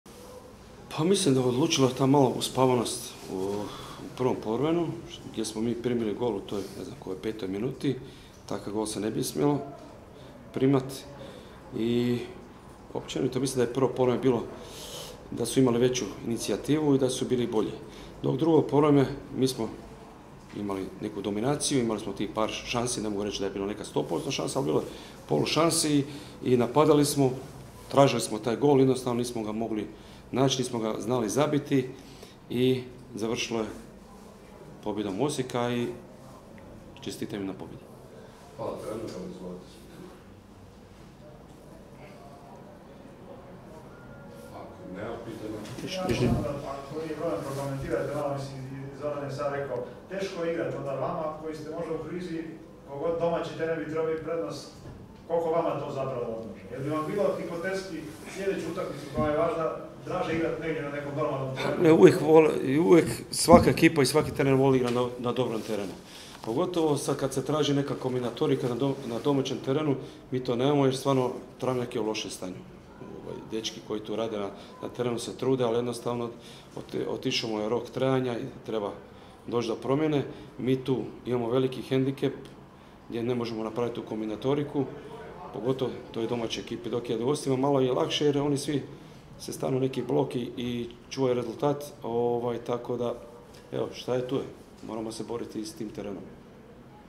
Trener Roy Ferenčina nakon utakmice s Osijekom
Trener Slaven Belupa Roy Ferenčina komentirao je utakmicu Slaven Belupa i Osijeka u kojoj je naša momčad poražena rezultatom 0:1.

- Presudio je naš loš ulazak u utakmicu. Drugo poluvrijeme smo bili bolja momčad na terenu, ali na kraju moram čestitati Osijeku na pobjedi.

Cijelu izjavu pogledajte u videoprilogu